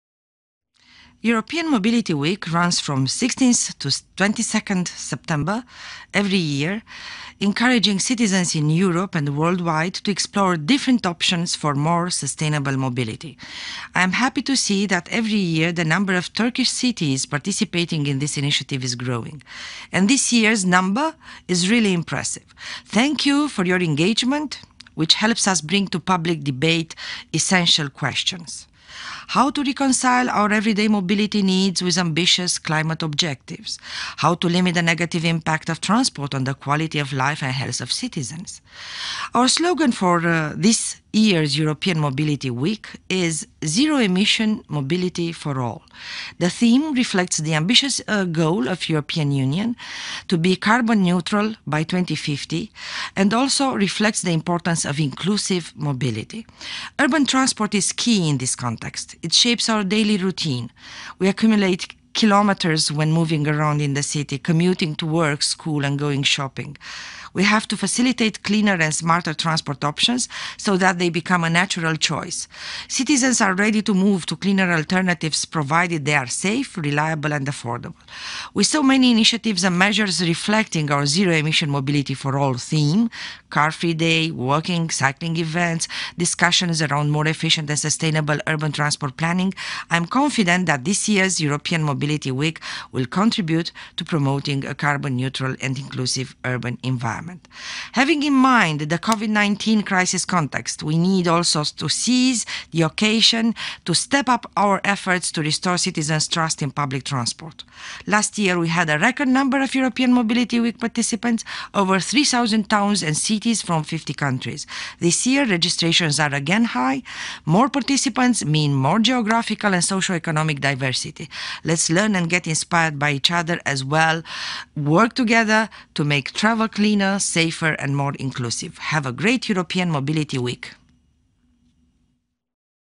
Ulaştırmadan Sorumlu Avrupa Komisyon Üyesi Adina Vălean'den, Avrupa HareketlilikHaftası mesajı
Ulaştırmadan Sorumlu Avrupa Komisyon Üyesi Adina Vălean, Avrupa HareketlilikHaftası çerçevesinde Türkiye kamuoyuna özel bir mesaj gönderdi!
Türkiye’ye yoğun katılımı için teşekkür ederken, bu senenin teması 0emisyonluHareketlilik sürecinin neden önemli olduğunu anlatıyor.